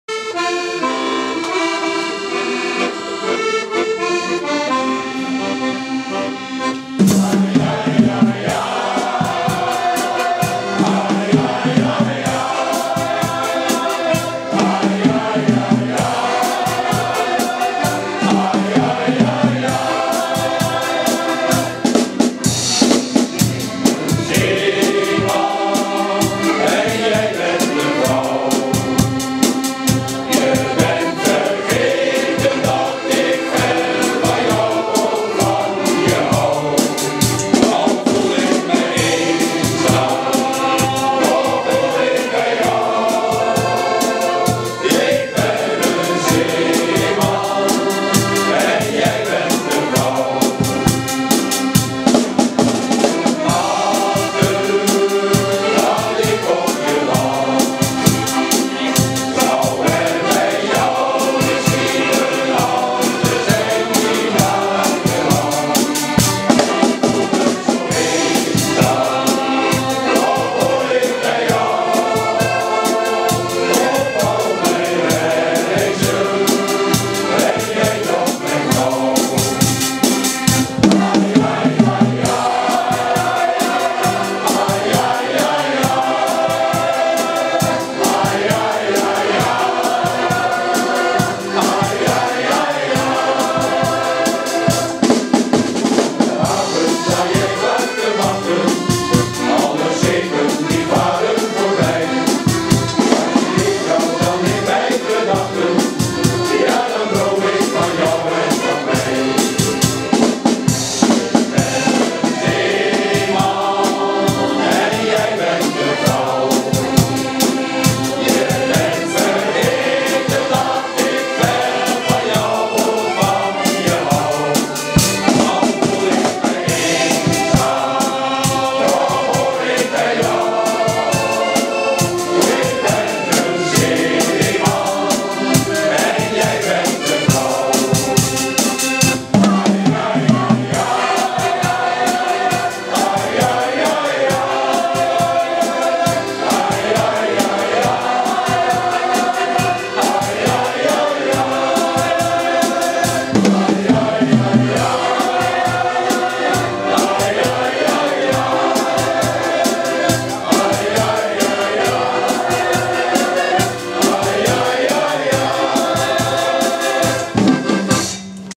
Meppel, Meppeler Muiters, De Poele, 7 nov 2009
Uitvoering van Meppeler Shantykoor: Meppeler Muiters , zaterdagavond 7 nov. 2009 in De Poele te Meppel